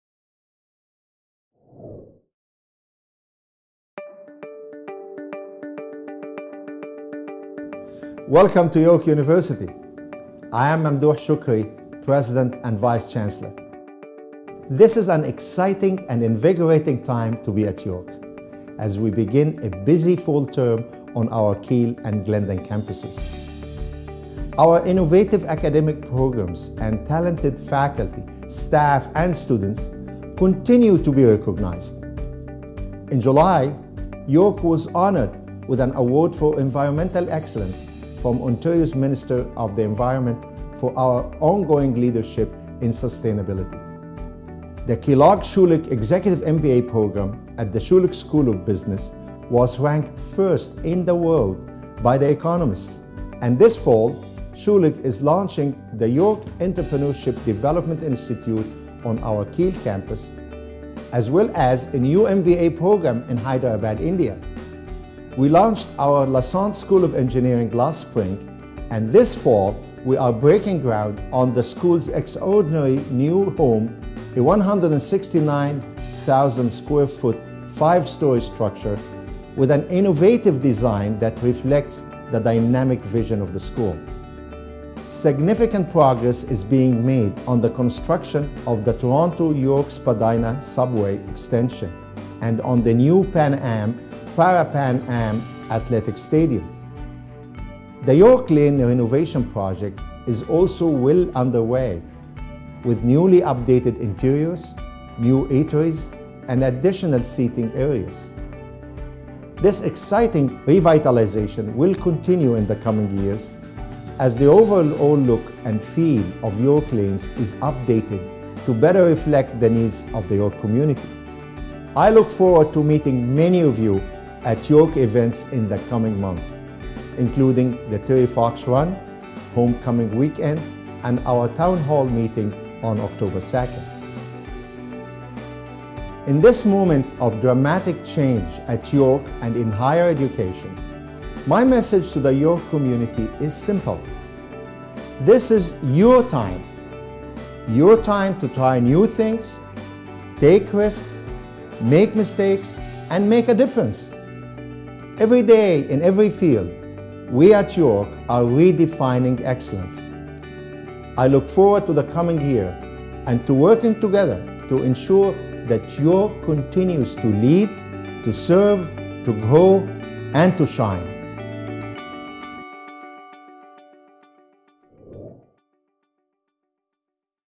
President's Welcome Message: Fall 2013
York University President & Vice-Chancellor Mamdouh Shoukri welcomes the York community to the start of the new academic year.